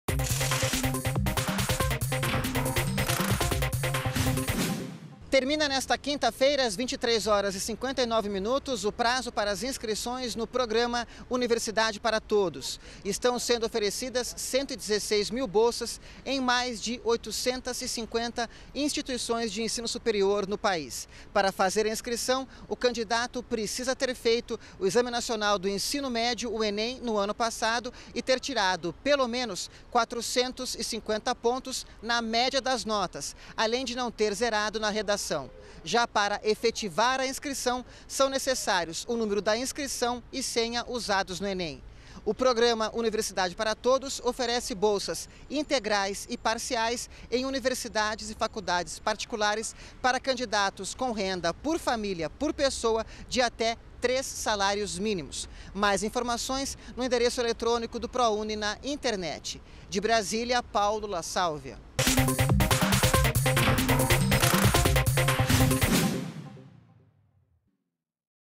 Prazo para as inscrições do Prouni termina nesta quinta-feira
ÚLTIMAS - 18.06.15: Termina nesta quinta-feira (18), às 23h59, o prazo para as inscrições do Programa Universidade para Todos (Prouni). Estão sendo oferecidas 116 mil bolsas em mais de 850 instituições de ensino superior. Para fazer a inscrição, o candidato necessita ter feito o Exame Nacional do Ensino Médio (Enem) de 2014. Saiba mais em prouniportal.mec.gov.br.